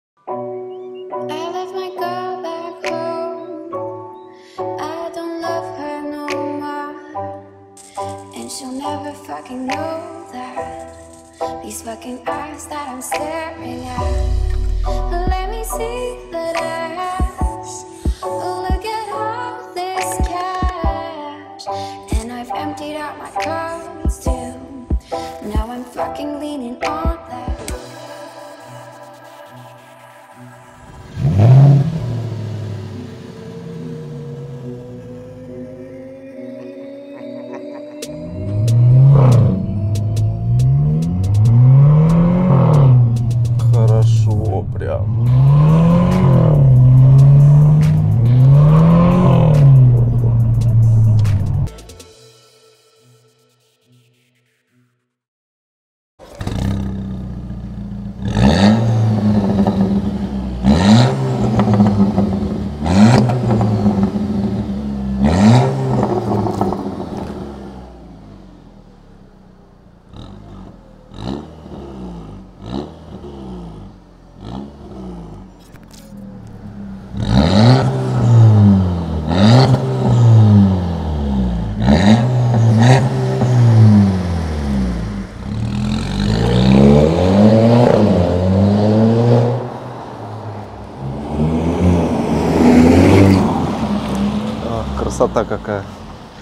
Регулируемый выхлоп Audi A5 2.0 | КАК ВАМ?
Audi A5 провозглашает своими основными принципами элегантность и мощь💪Преимущество этого автомобиля в том, что оно позволяет органично совместить спортивность, комфорт и сочные звуки выхлопной системы🔉

✅ Приехав к нам в ultravihlop мы прокачали штатную выхлопную систему и установили вакуумную заслонку с комплектом управления выхлопом🤌 Удалили штатный резонатор и доработали громкость звука в процентом соотношении.

🚩 Адрес: Москва, проспект Вернадского, 12Д